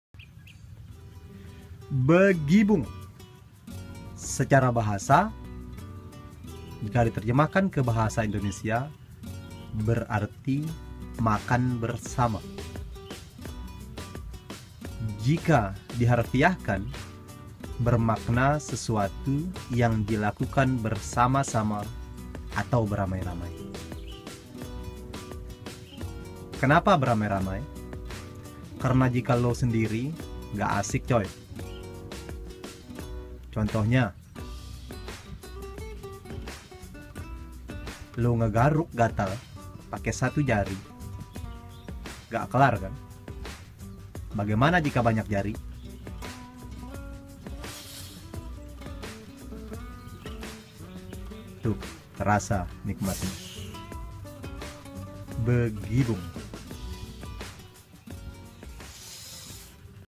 Apa itu Begibung?
Lihat yang dilkukan orang ini, memperagakan contoh sederhana dari makna kta begibung
Wow